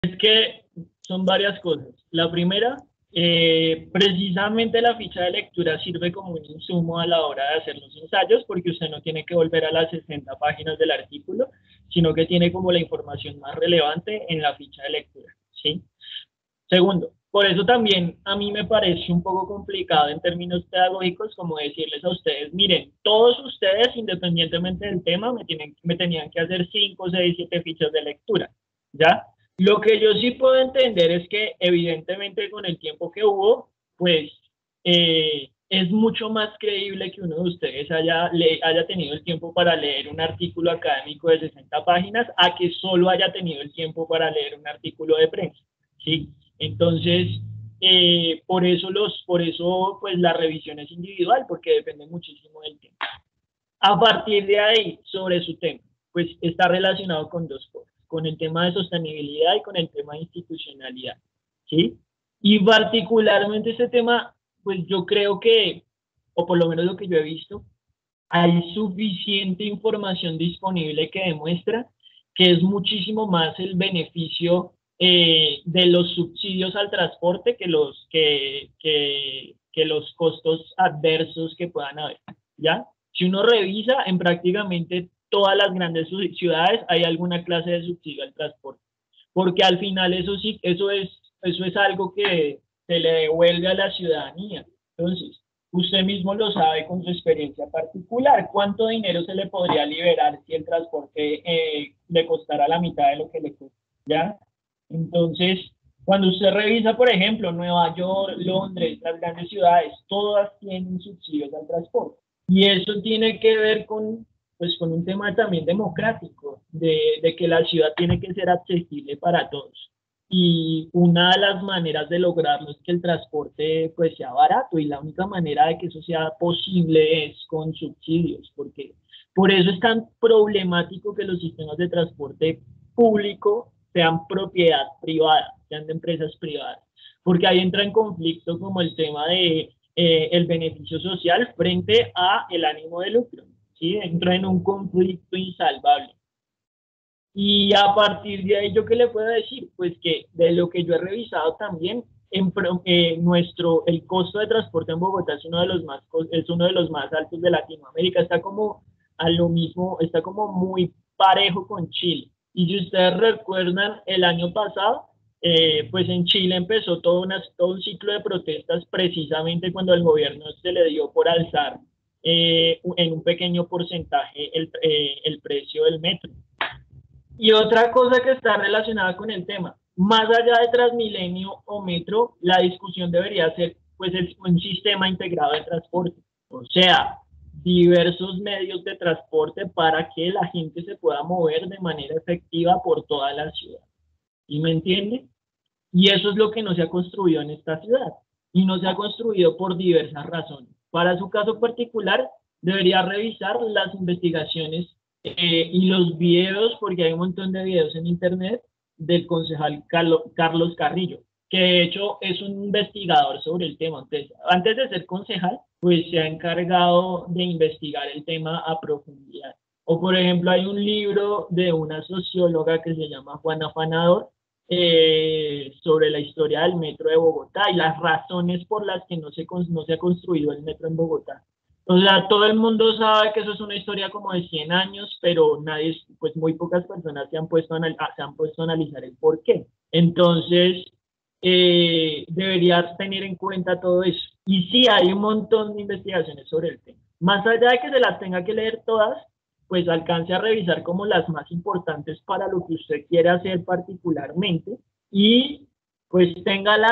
Fragmento de nuestra clase del 29 de Agosto del 2020 - Movilidad sostenible
Hablamos de los problemas de movilidad en la ciudad relacionados con la desigualdad social.